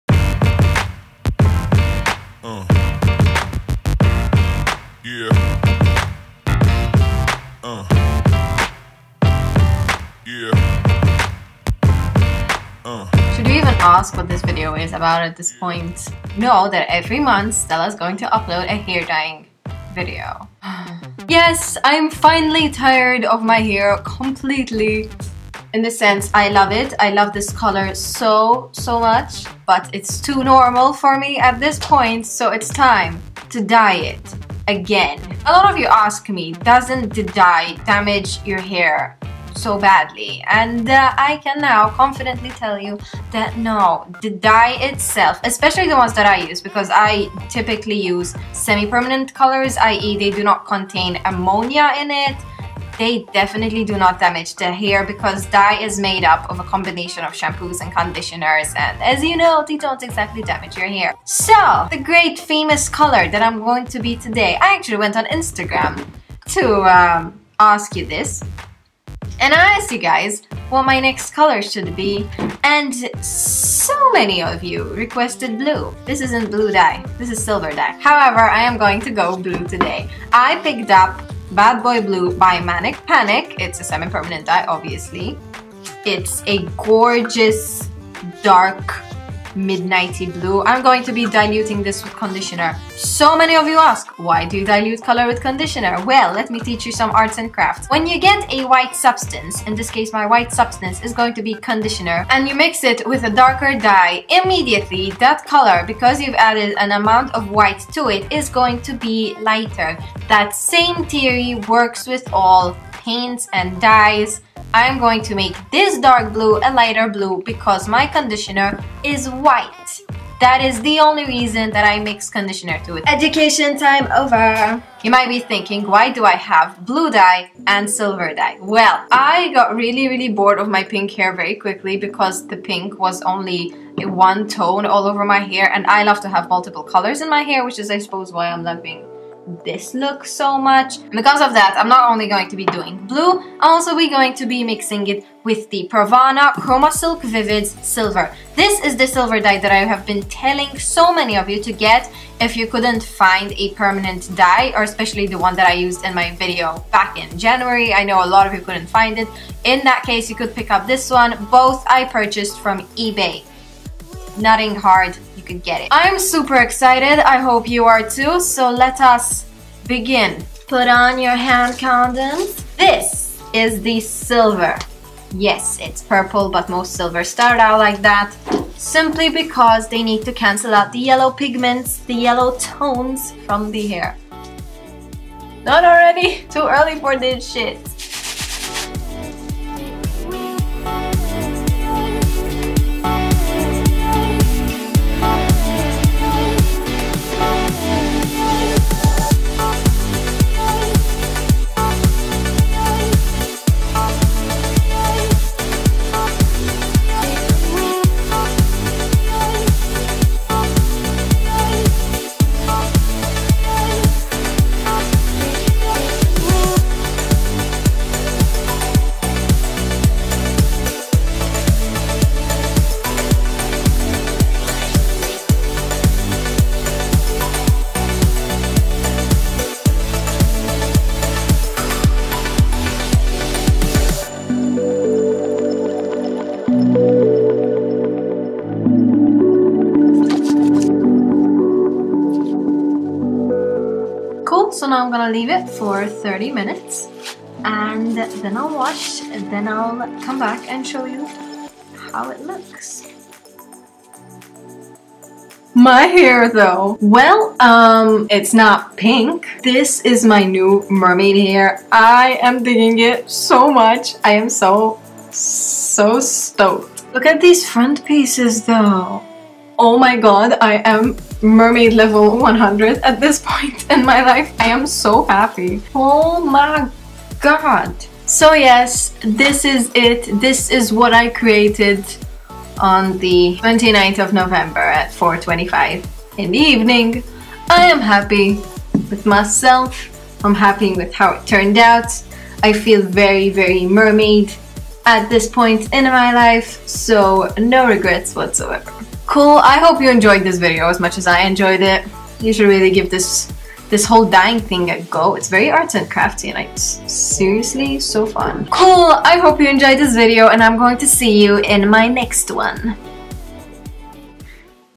How To: BLUE SILVER HAIR COLOUR | Stella
GALAXY\ AURORA BOREALIS HAIR COLOUR WHATTTT.
yahaass.

One of the dopest looks I've ever had to honour of trying cos like its beautiful. To me anyway.

Colours used:
MANIC PANIC Bad boy Blue diluted with white conditioner
PRAVANA VIVIDS Silver

That's all basically.

PS: IF YOU INTEND TO USE SILVER ON YOUR HAIR, YOU NEED VERY LIGHT HAIR. I'm sorry but thats the only way to achieve it really 😭

If you have any questions, leave them down below!

XOXO

Stella

stellacini